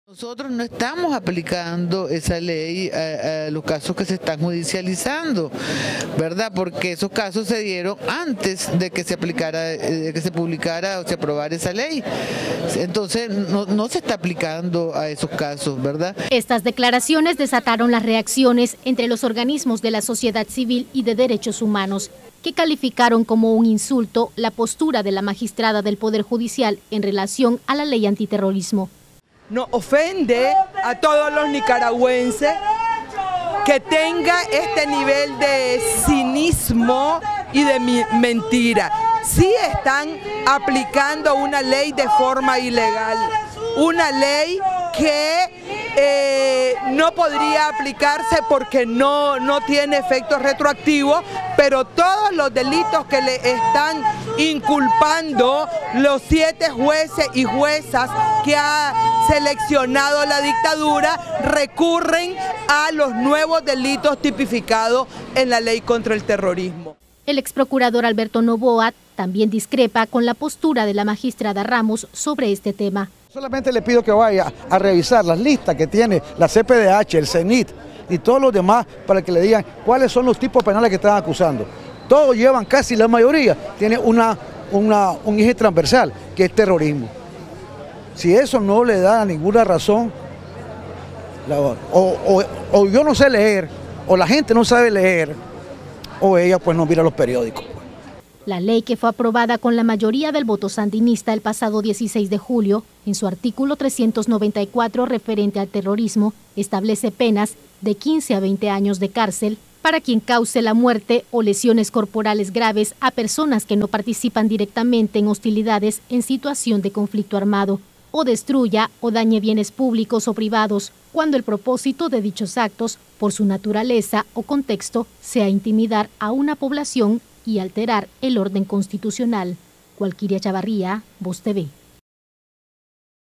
"La magistrada miente", aseguran organismos y expertos, sobre Ley antiterrorista
Miembros de diversos organismos de la sociedad civil, derechos humanos y expertos jurídicos reaccionaron a las declaraciones de la magistrada Alba Luz Ramos y al comunicado que ha publicado el Poder Judicial sobre la aplicación de la Ley 977, Ley antiterrorista.